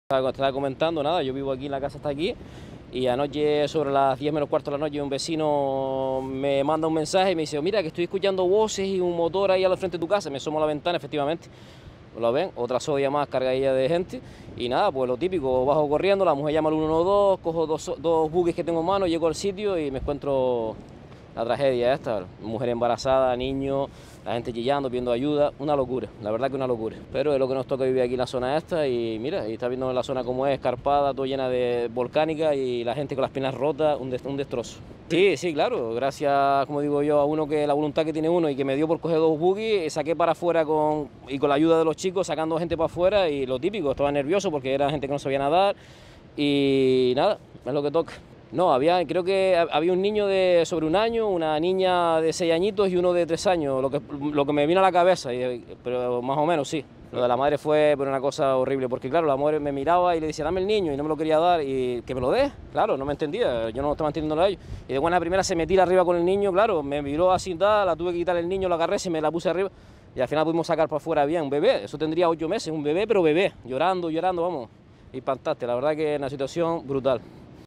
Los vecinos de Órzola (Lanzarote), vuelven a ser héroes al rescatar en el naufragio de la patera
Marcos Lemes, vecino de Órzola (Lanzarote), cuenta como fue el naufragio de la patera de anoche y como acudió a ayudar en las labores de rescate.
En el mes de noviembre del año pasado volvió a producirse en esa misma zona un naufragio similar y los vecinos también demostraron su valentía al ayudar desde el primer momento a rescatar a los supervivientes, al igual que hicieron anoche